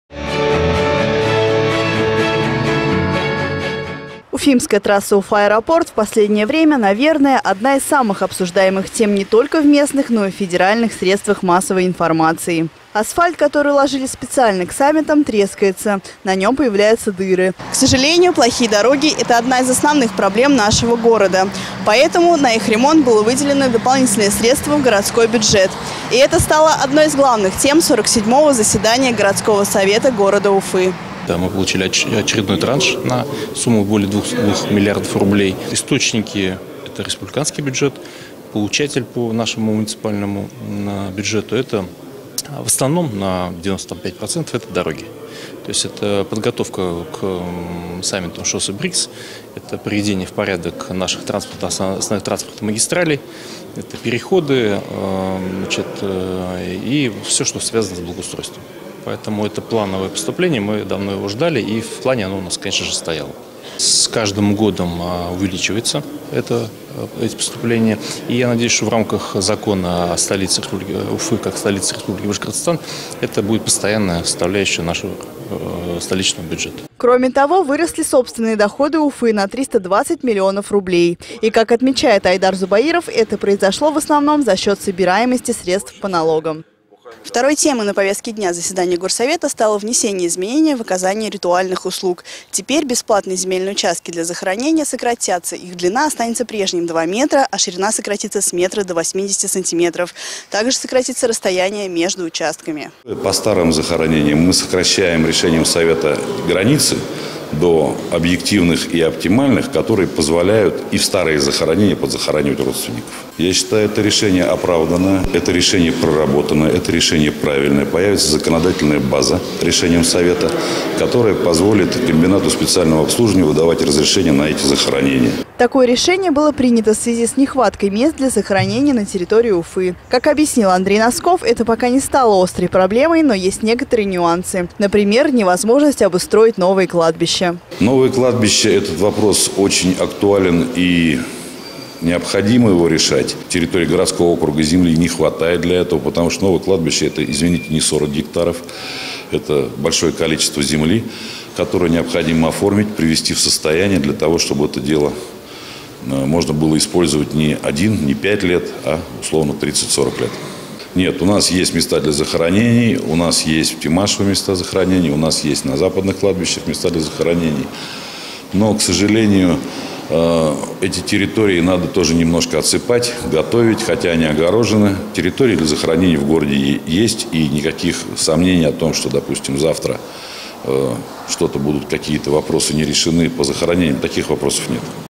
На ремонт и содержания дорог Уфы республика выделила 2 миллиарда рублей
Плохие дороги - это одна из основных проблем нашего города. Поэтому на ее решение были выделены дополнительные средства в городской бюджет. И это стало одной из главных тем сорок седьмого заседания Городского совета города Уфы